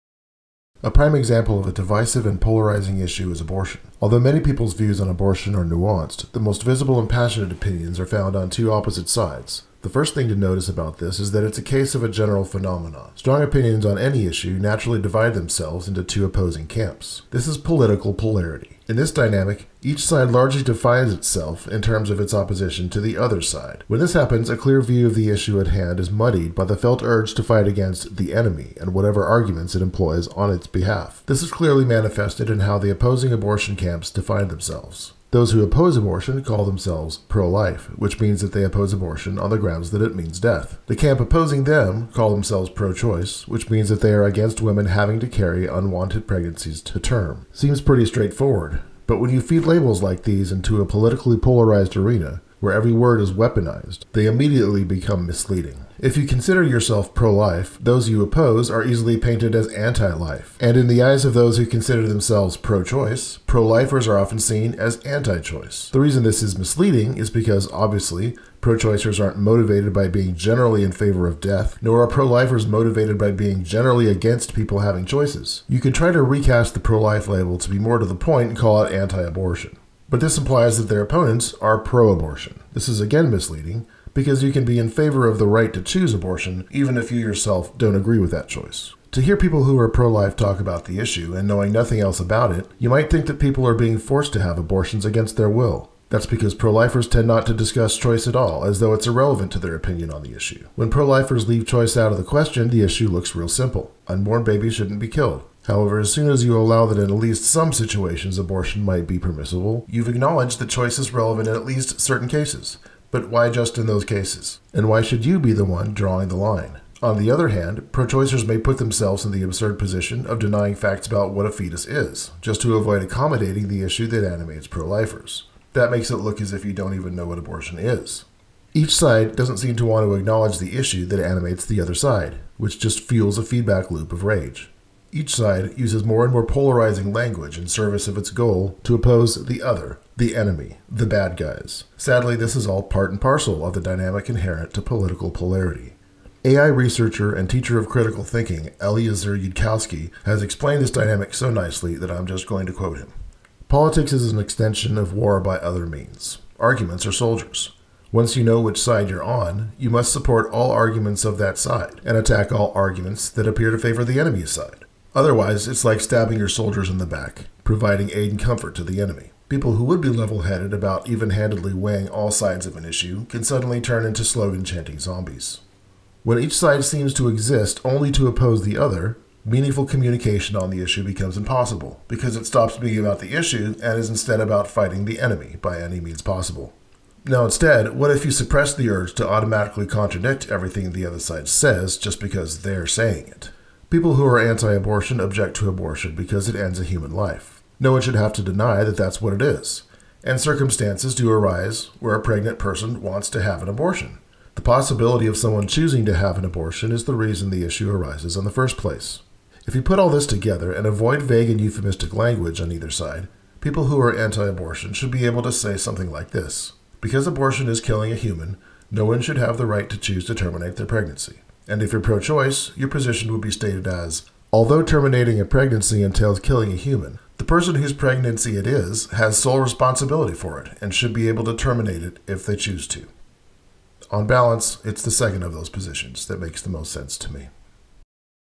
How (not) to Think about Abortion
Yudkowski - "Politics Is the Mind-Killer"
Holding two seemingly contradictory thoughts at the same time can be a source of wry comedy...